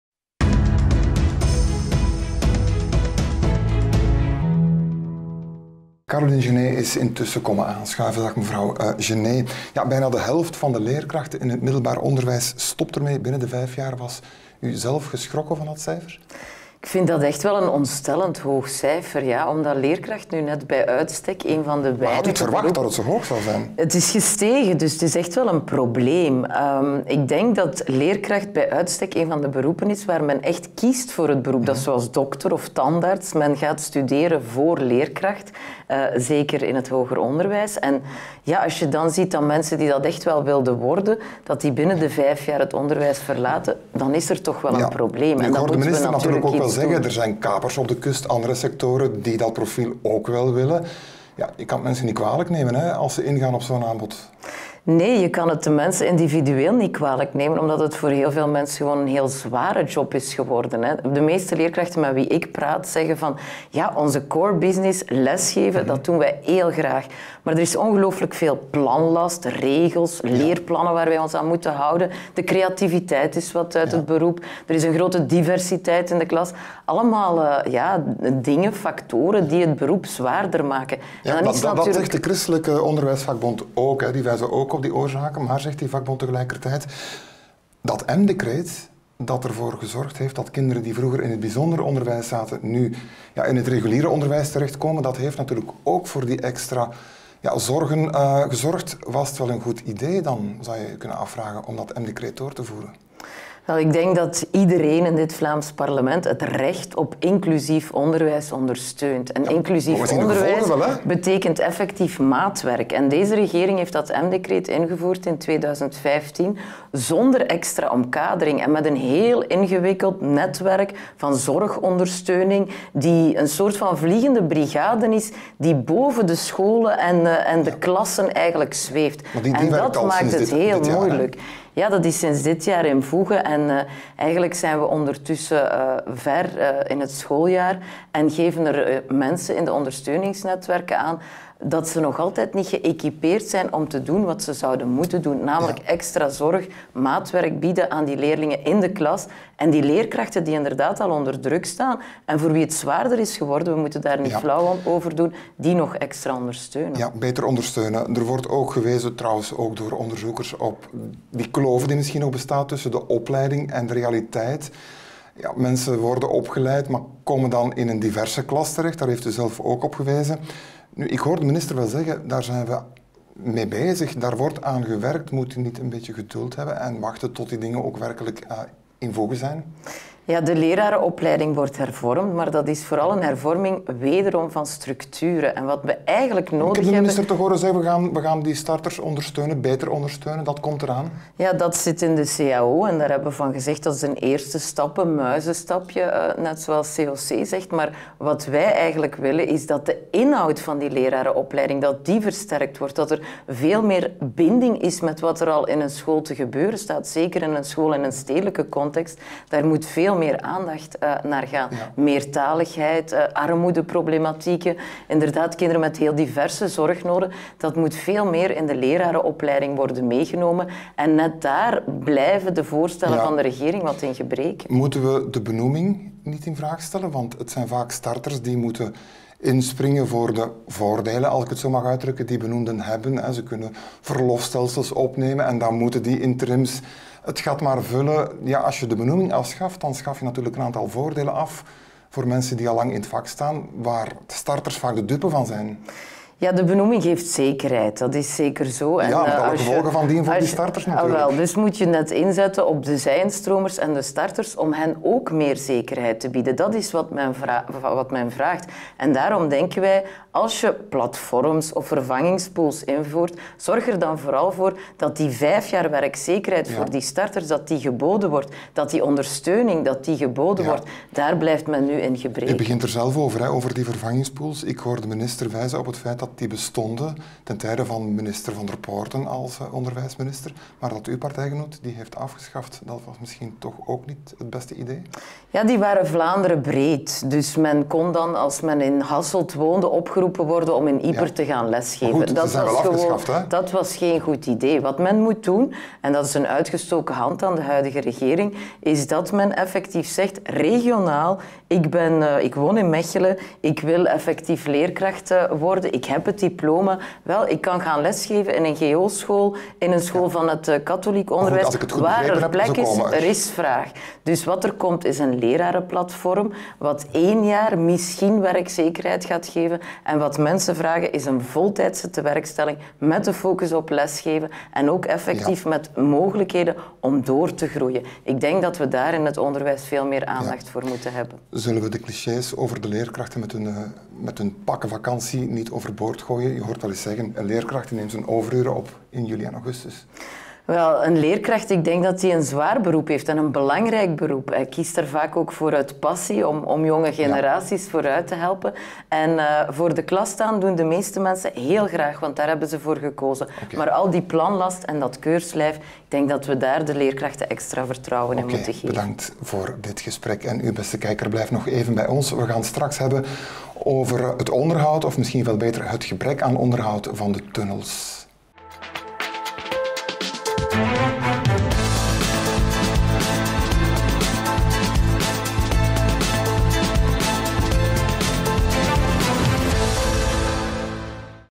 Studio Vlaams Parlement: Caroline Gennez over uitstroom van jonge leerkrachten
Bijna 50 procent van de beginnende leerkrachten in het secundair onderwijs verlaat de klas binnen de vijf jaar. In het basisonderwijs ligt het aantal lager. Daar gaat het om bijna 25 procent. Volgens Caroline Gennez (sp.a) moet de regering dringend bijkomende maatregelen nemen om de werkdruk voor leerkrachten te laten dalen.